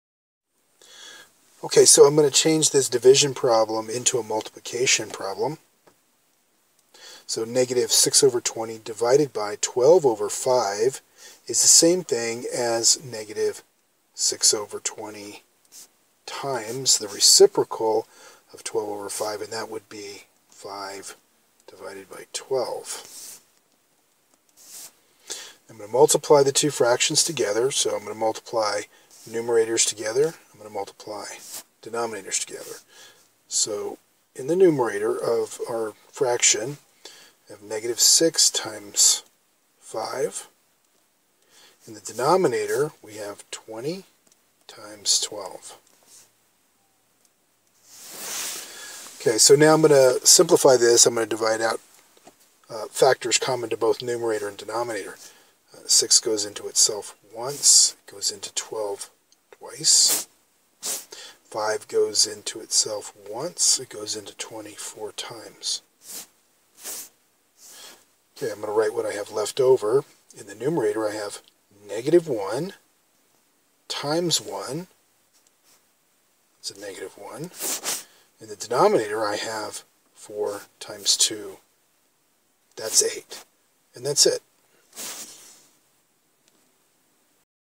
Dividing Fractions -6/20 divided by 12/5. Youtube
To divide two fractions, multiply the first fraction by the reciprocal of the second fraction.